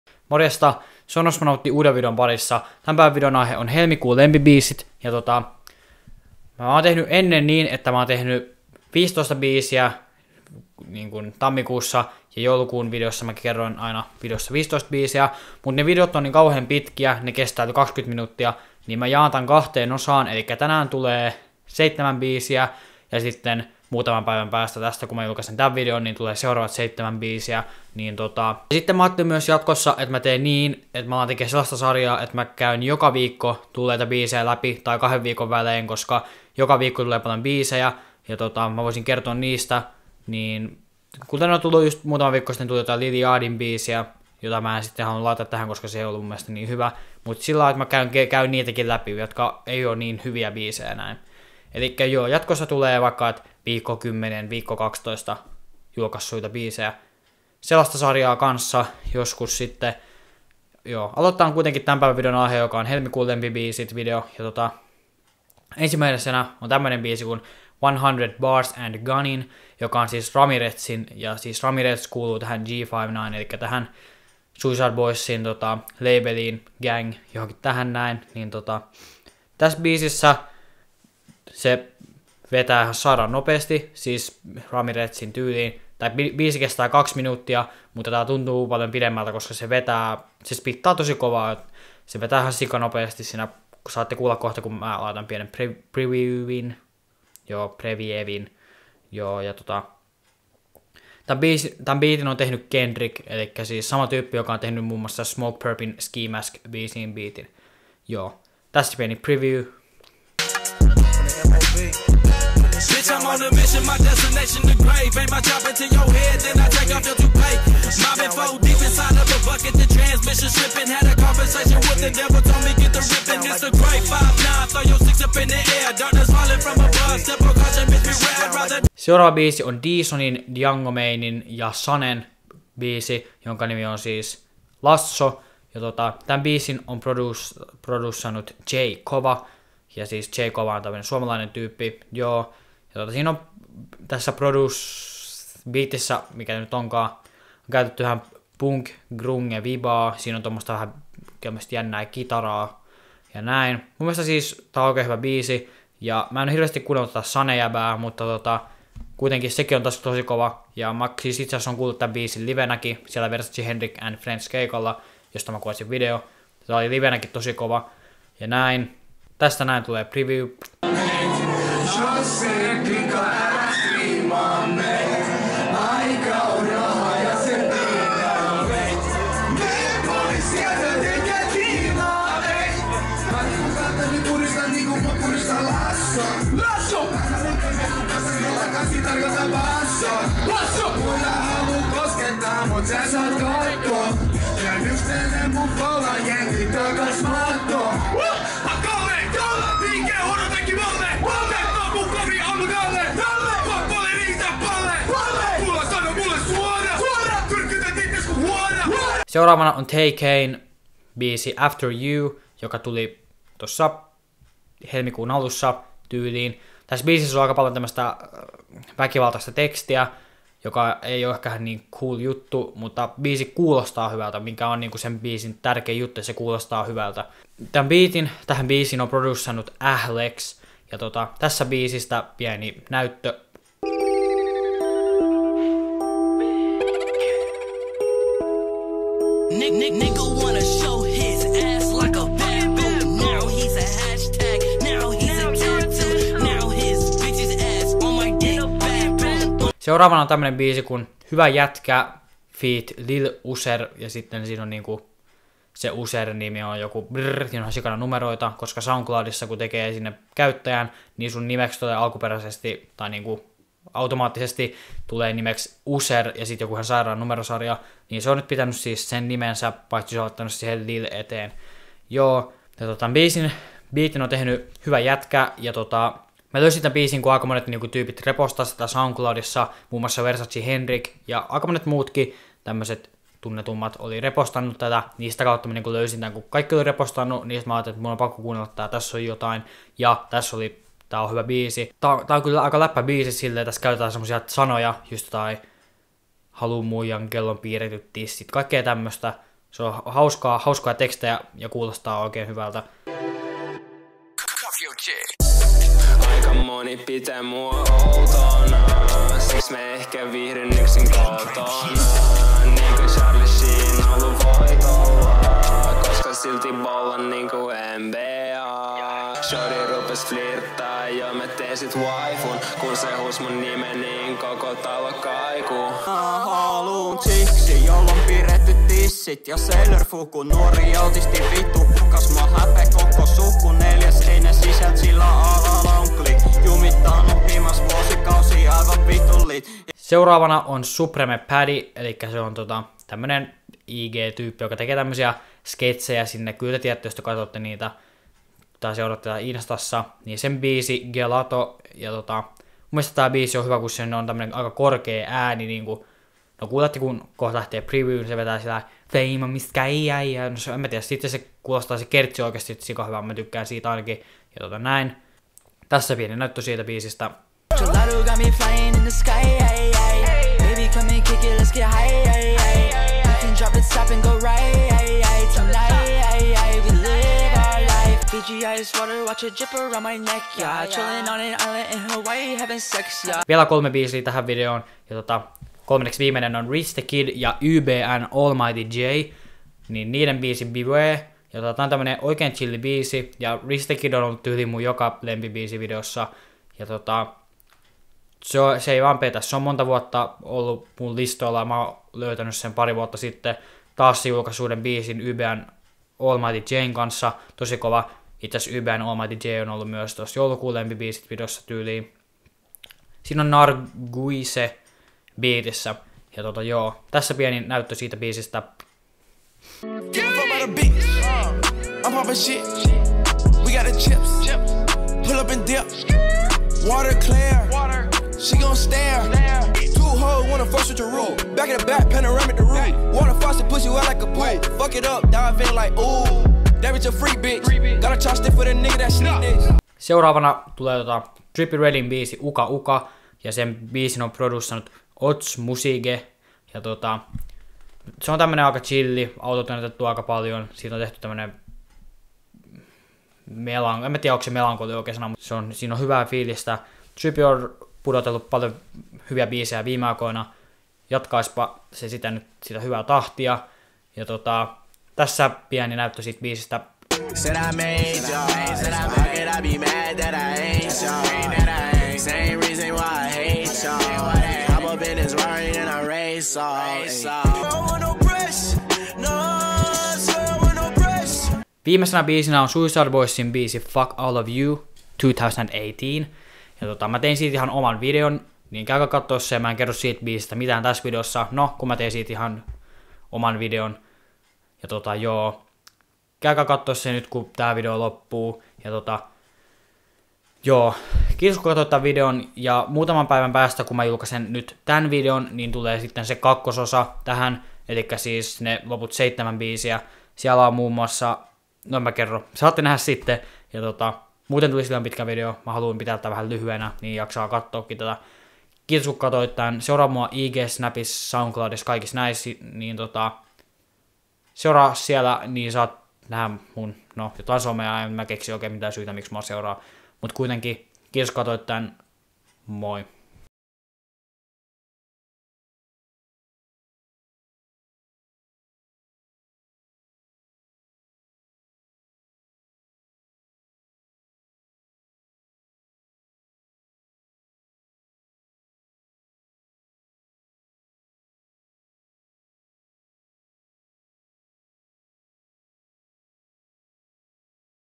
HELMIKUUN LEMPIBIISIT 1/2 ( Tay-K, Trippie Redd..)
tänään tälläi video mis helmikuus tulleita/helmikuus paljon kuunnelluit biisei multa! huomen osa 2! rakastan sua 💜💜 sano se takasin 💘 jee